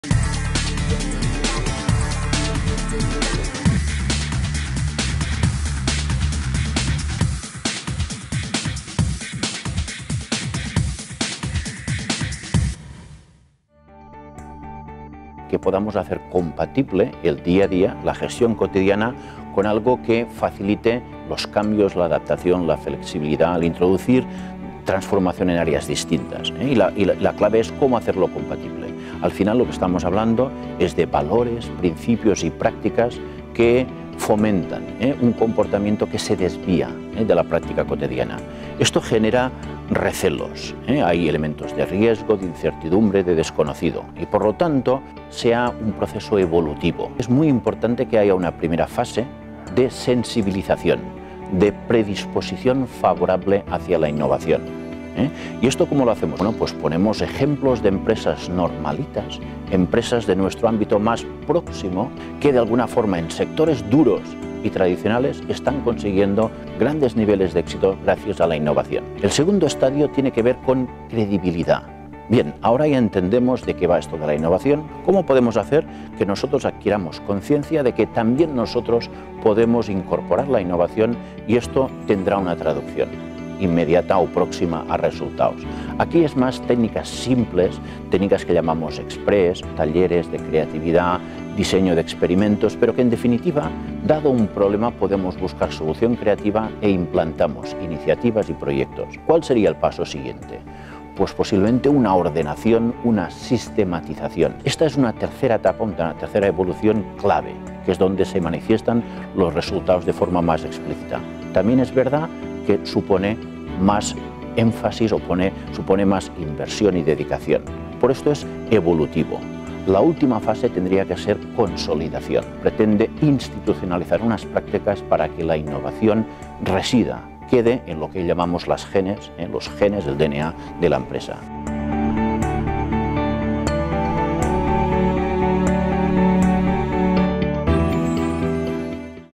La cultura innovadora en la empresa
El profesor del IESE Joaquim Vilà explica en este vídeo cómo se debe articular una cultura de innovación en las empresas que quieran aplicar cambios y adaptarse a los nuevos tiempos.